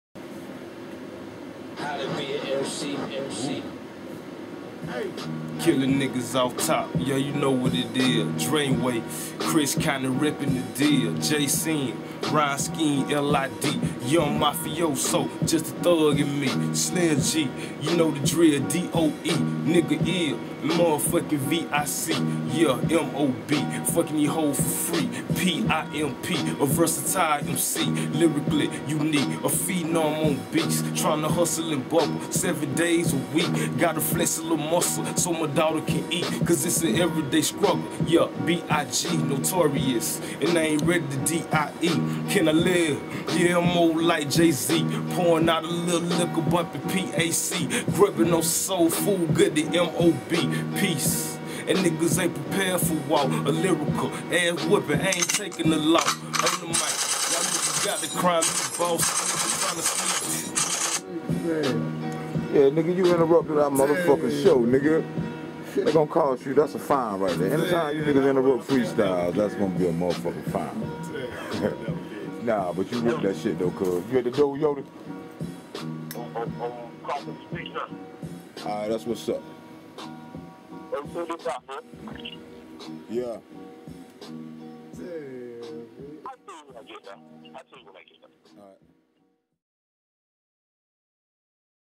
CAMP L.I.D. PRESENTS J-SIN FRESTYLE
FREESTYLE BY J-SIN(JASON WALKER), CAMP L.I.D. SOLDIER AND MEMBER OF YUNG MAFIOSO. MIXTAPE COMING SOON